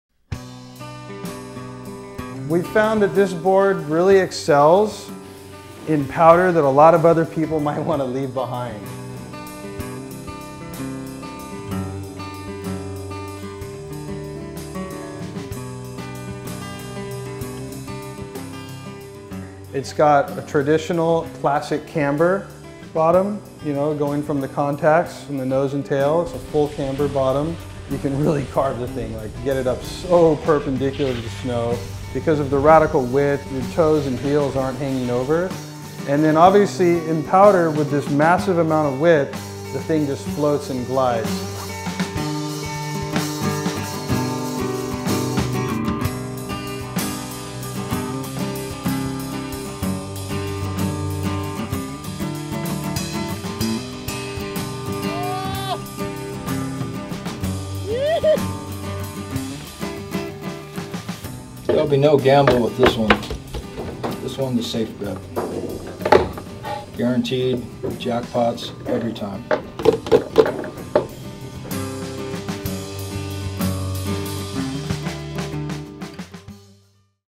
JAMIE LYNN SHORT WIDE | 2022-2023 LIB TECH SNOWBOARD
Baldface dreaming, resort ripper… Jamie went a little longer and a little narrower on his favorite pow blaster this year and now it rips the hardpack as well as the pow. Wide enough to  oat effortlessly through tight trees, loves a straight line, glides through the flats and snaps tight pockets. Perfect for pow days at home or that dream cat trip.

VOLUME SHIFTED, RIDE THIS BOARD SHORTER THAN YOUR NORMAL BOARD. 648 SQUARE INCH SURFACE AREA.

AMAZINGLY EASY RIDING SHORT WIDE FREERIDER
RIPS THE RESORT, BLASTS THE POW
MAXIMUM FLOAT AND PLANING SURFACE (648 sq. in.)

ART BY: Jamie Lynn (@jamiemlynn)
“The Short Wide is my go to board for anything deep. I love the nimble quality of the size and weight of the board. It provides the flotation and buoyancy of a board that would be in the 160cm or greater range. But to have that offered in a 150 to me seems like a logical choice.” - Jamie Lynn